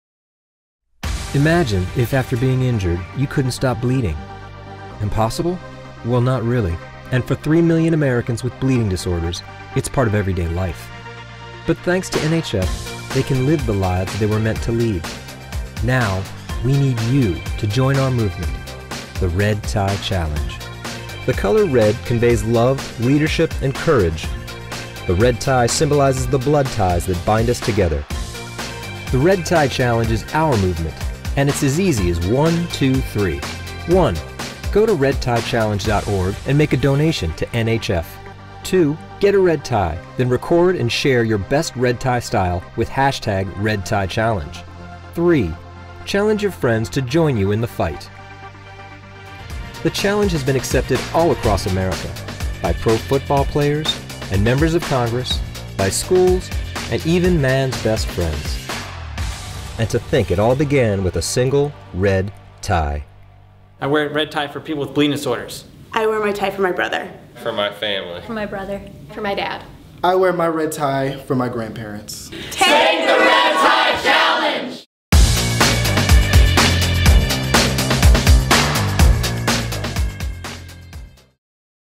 Red Tie Challenge: Consider Yourself Challenged!
A movement for the bleeding disorders community started with one red tie. Grab a red tie and take the challenge!